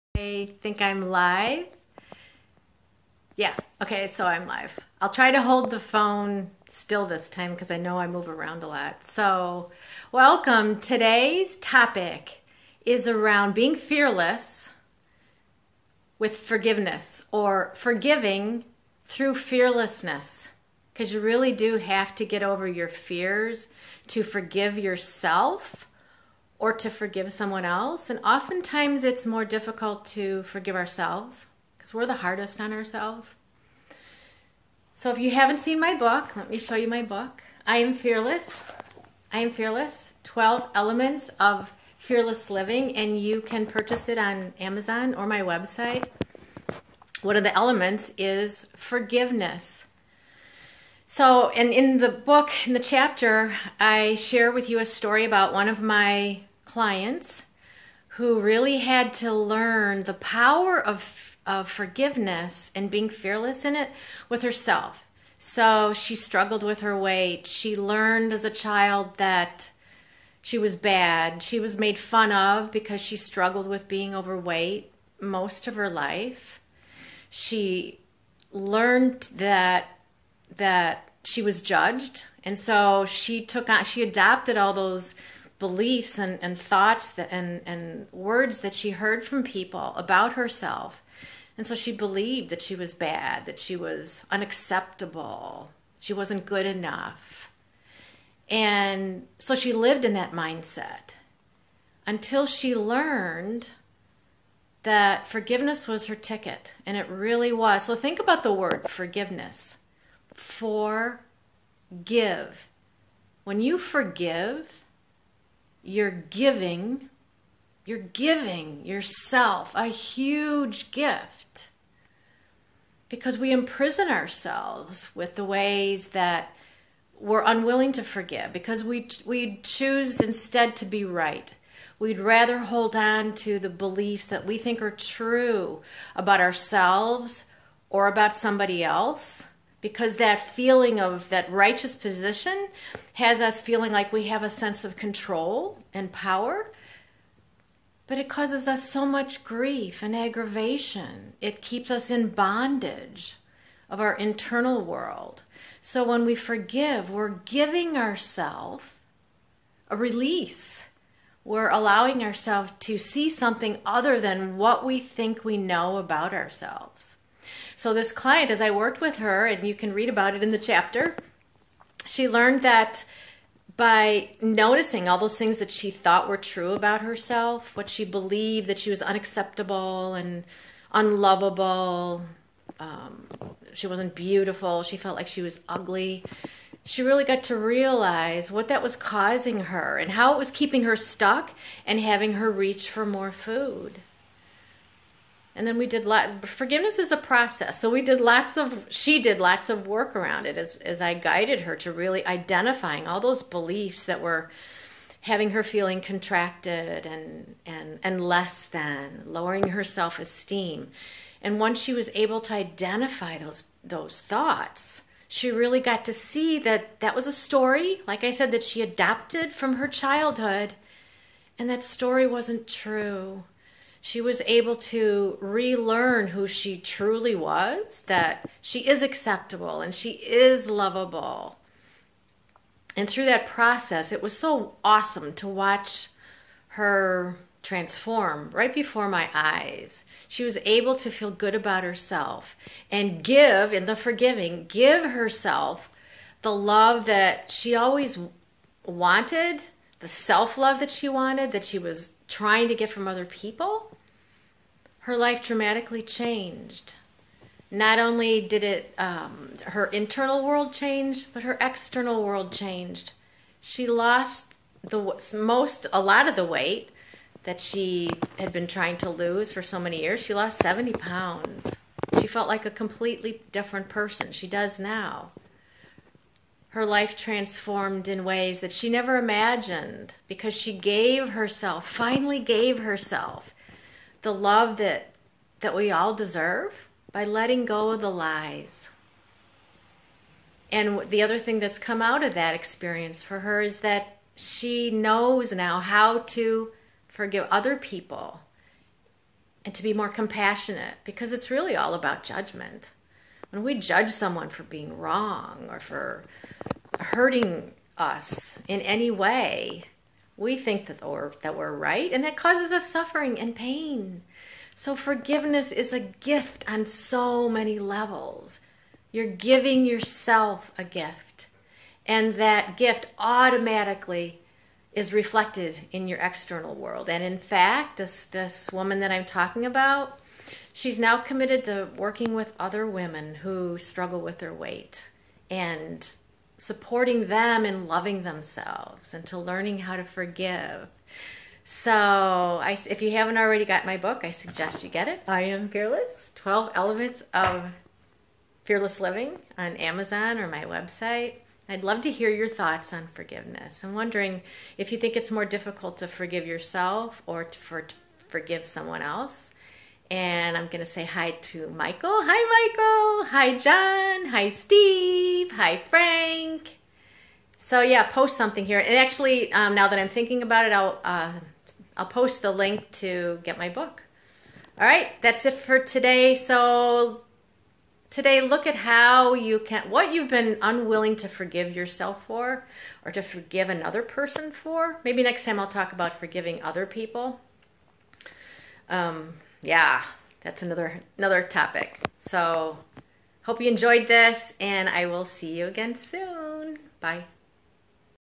Forgiveness Requires Fearlessness
To truly forgive means you have to let go of what you think is true about yourself and let it go.